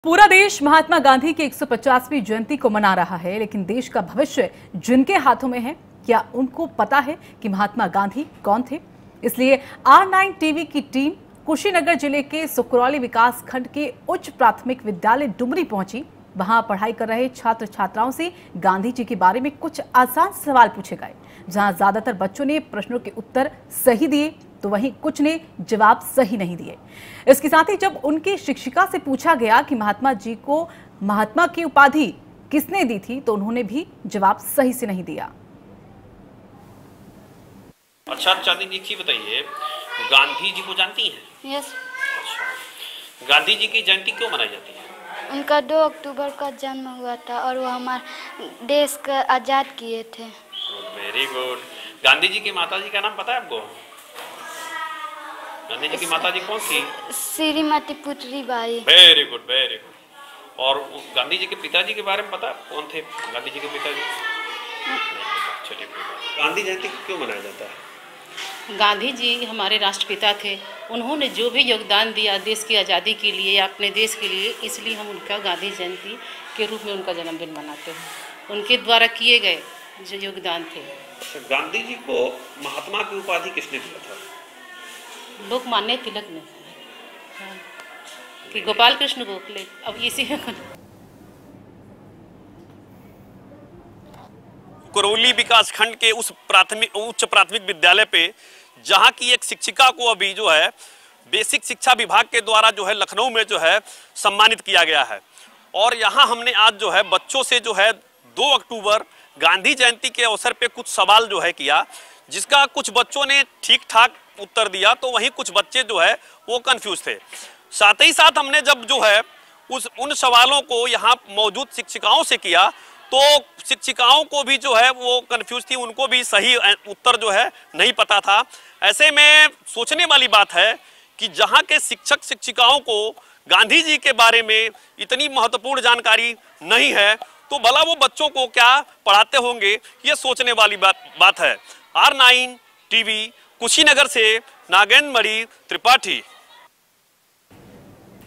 गांधी की 150वीं जयंती पर छात्र-छात्राओं से गांधी के बारे में पूछे सवाल
R9 TV की टीम ने गांधी की 150वीं जयंती पर कुशीनगर के उच्च प्राथमिक विद्यालय में पढ़ाई कर रहे छात्र छात्राओं से गांधी जी के बारे में कुछ सवाल पूछे गए...